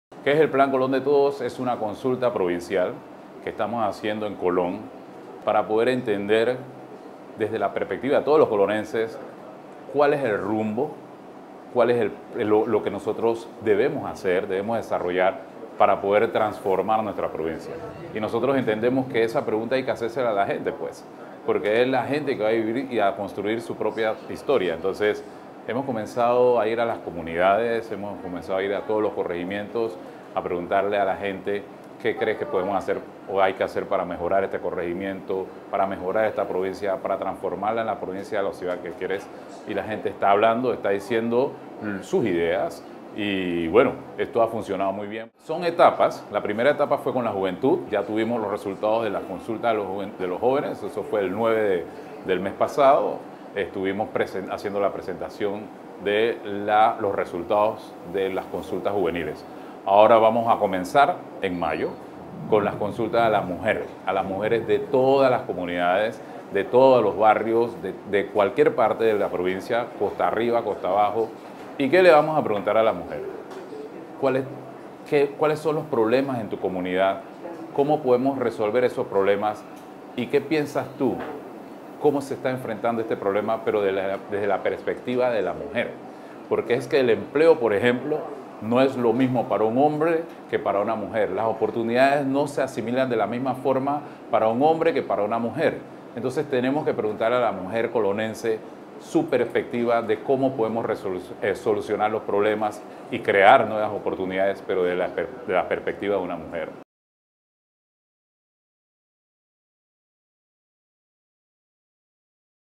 Colonenses trabajan en la creación de plan integral para transformar la provincia
La consulta ciudadana "Colon para Todos" promovida por el independiente, Diógenes Galván, busca trazar la hoja de ruta para el pueblo a 20 años